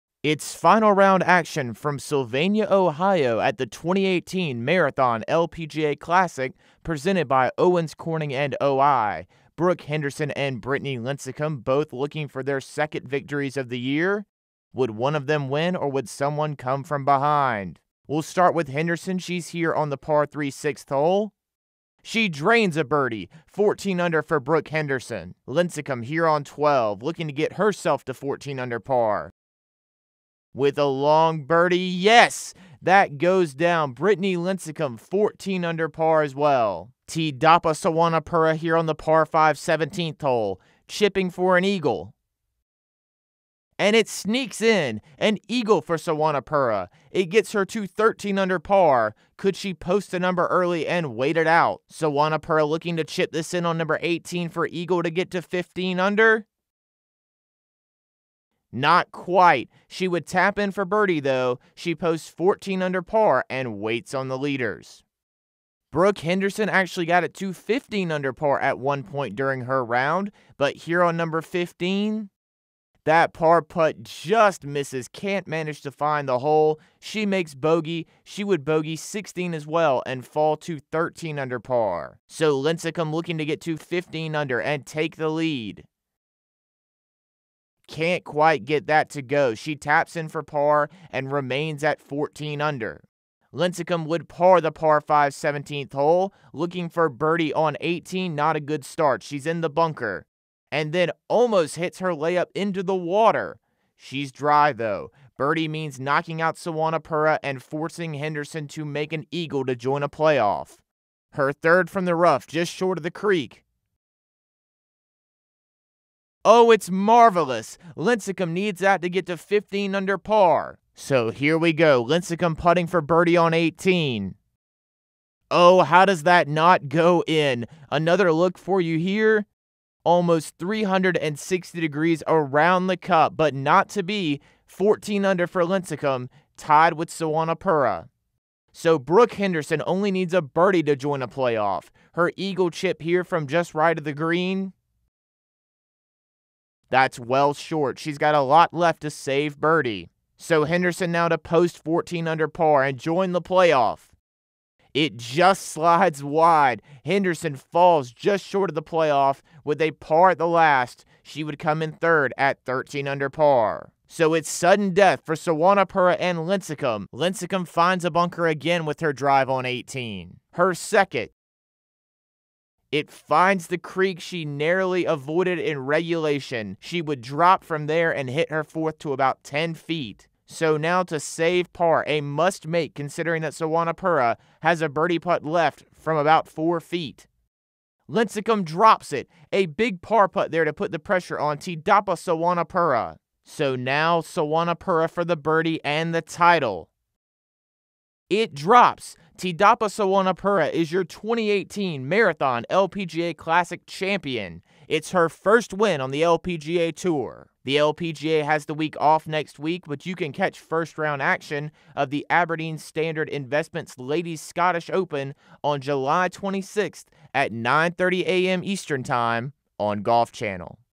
Final Round Highlights 2018 Marathon LPGA Classic
Thidapa Suwannapura shot a final round 65 (-6) to win the 2018 Marathon LPGA Classic Presented by Owens Corning and O-I in a one-hole playoff with Brittany Lincicome.